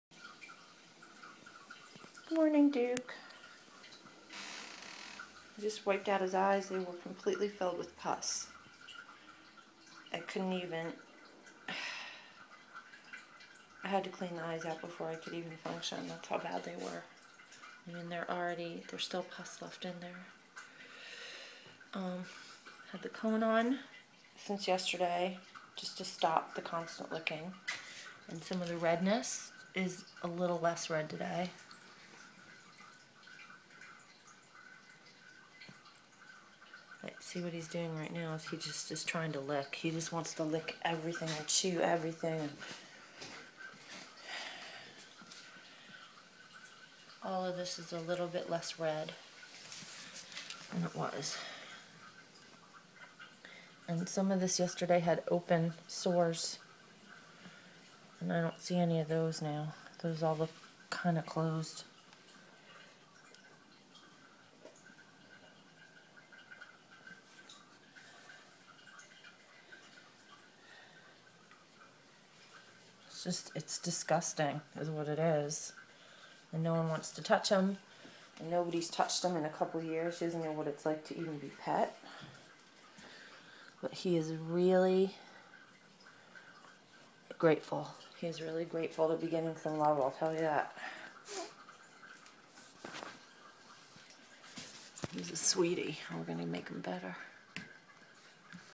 Duke_day 2
Poor Duke on day 2 of his rescue. He is being a champ with his cone on and is really suffering from his mange, infected eyes, and sores. He's being a sport. Tomorrow we see the vet.